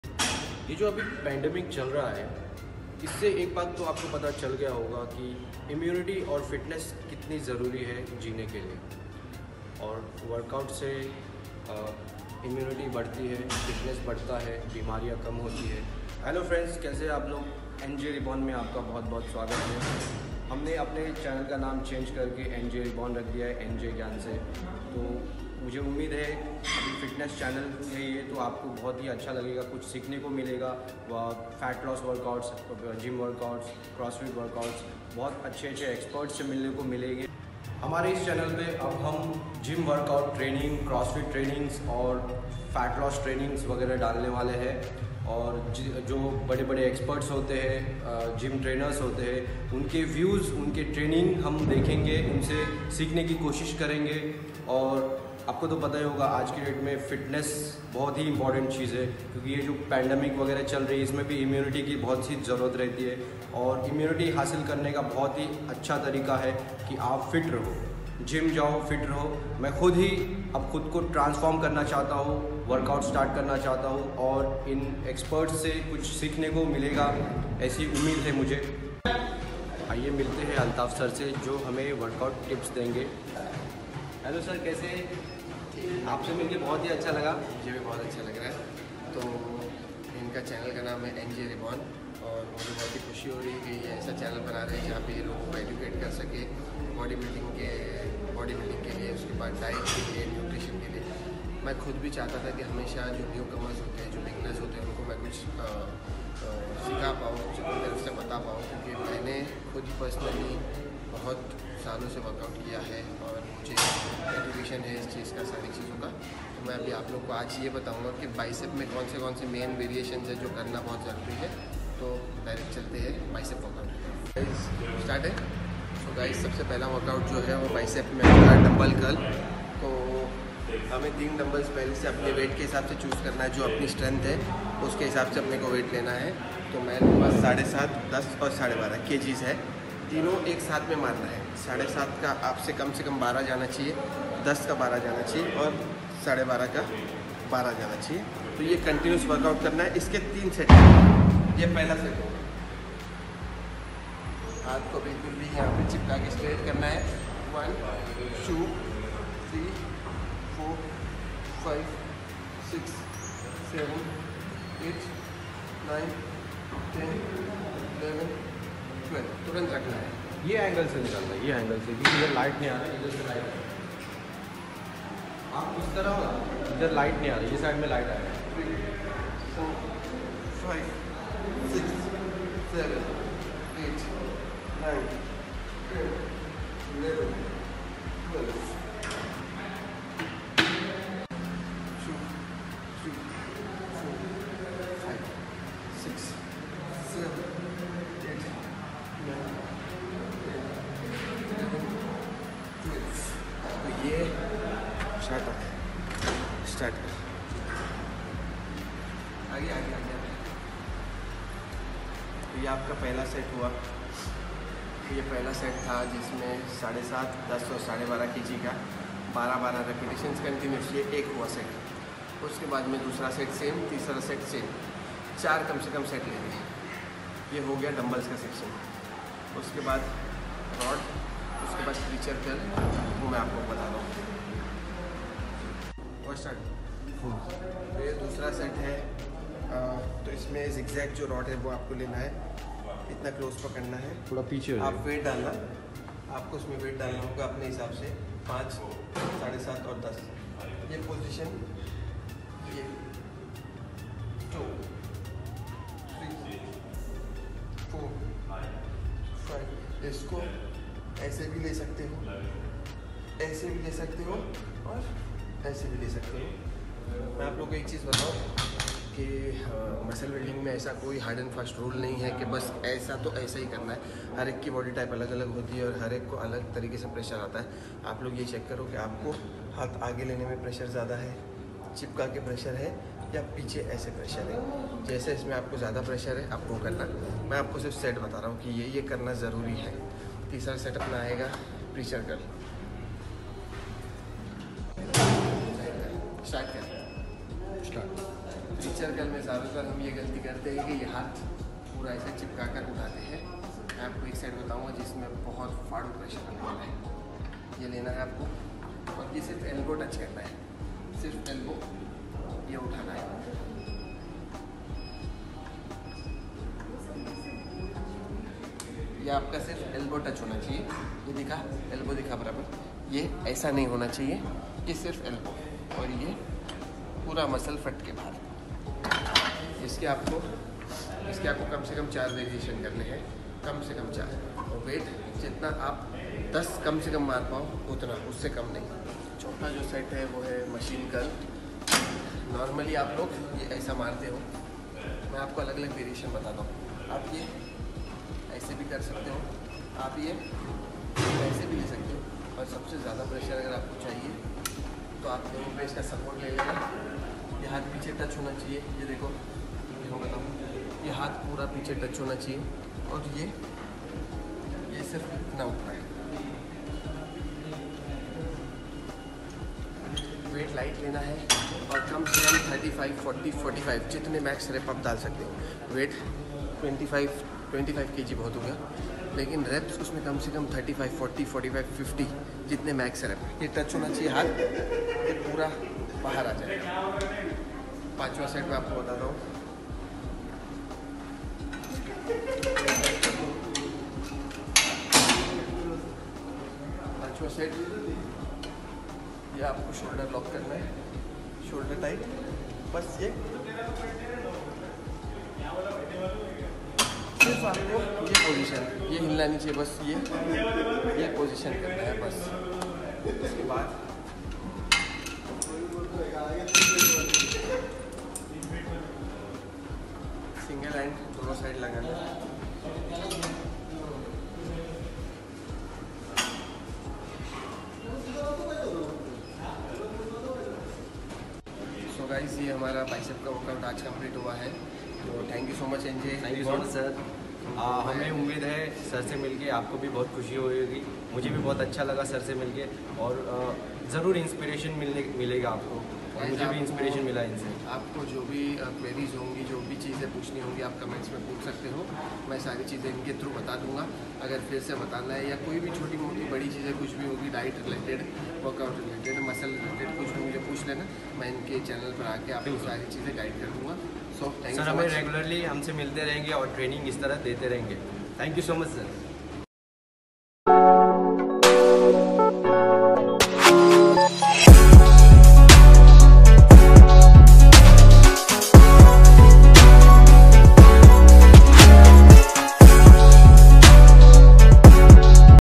Unique Biceps workout Training | Altaf Sir | NJ REBORN
Here in this video we got to learn unique technique of Biceps workout from Altaf Sir. We will come up with next video of  other body part's workout.
'NJ REBORN'. Thank you so much. Please keep Supporting.